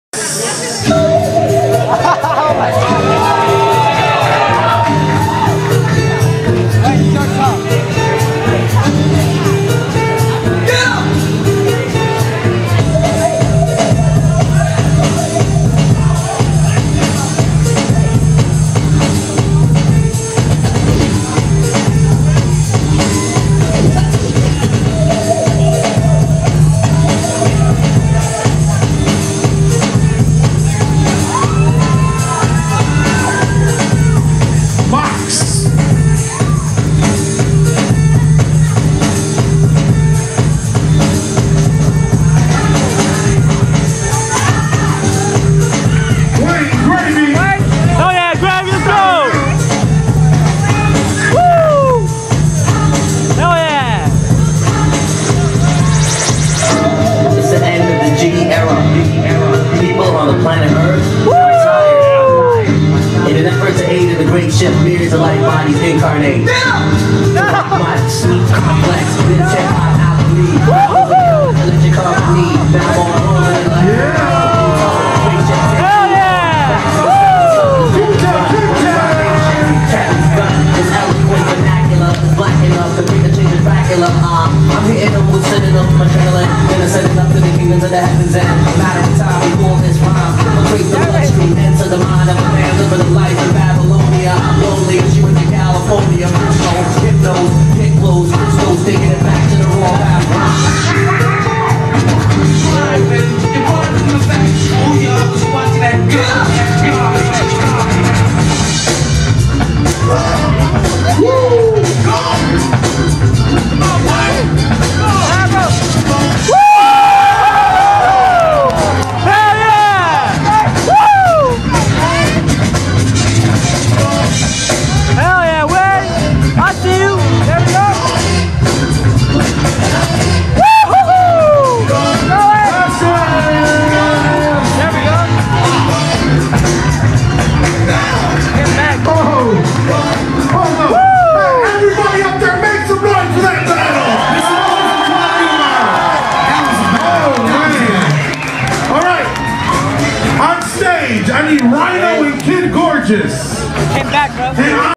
Moks (Ground Syndrome / Motion Disordez) vs Wayne (Stylin' Out Chicago Krew)
Bboy moks vs bboy Wayne at the Winter block party 1 vs 1 bboyJam. Chicago, IL